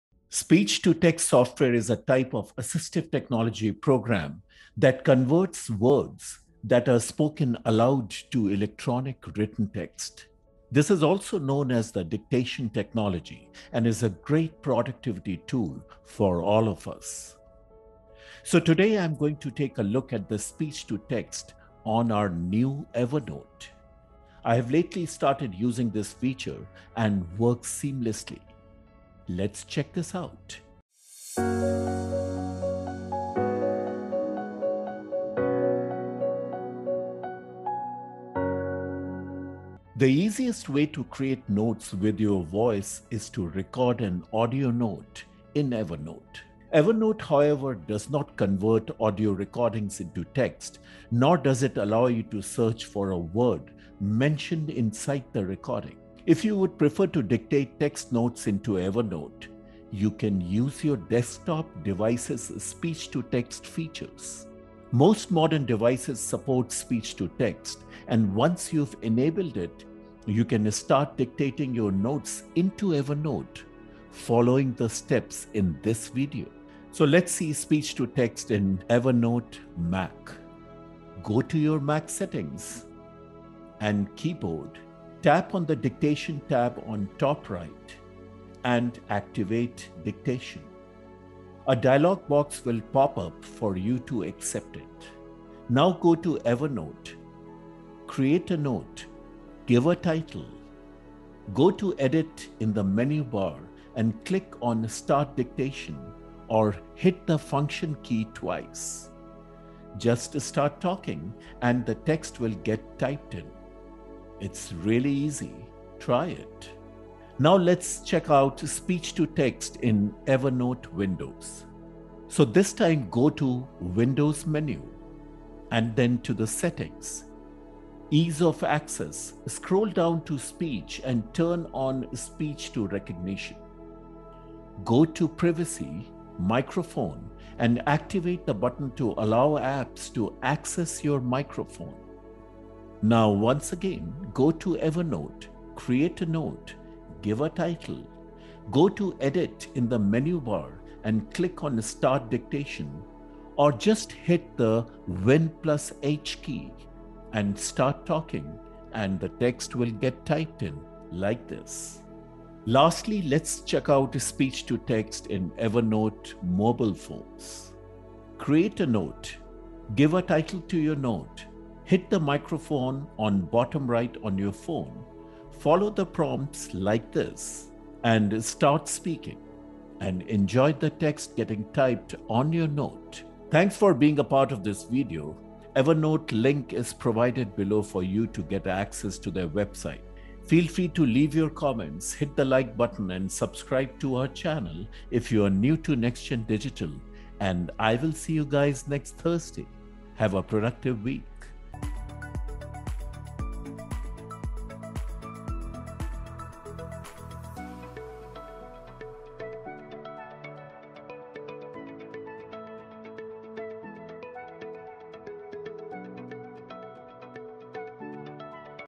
Dictate Notes Using Speech To Text On Evernote (Mac, Windows, Mobile)
If you'd prefer to dictate text notes into Evernote, you can use your desktop device's speech-to-text features. Most modern devices support speech-to-text and once you've enabled it, you can start dictating notes into Evernote following the steps in the video!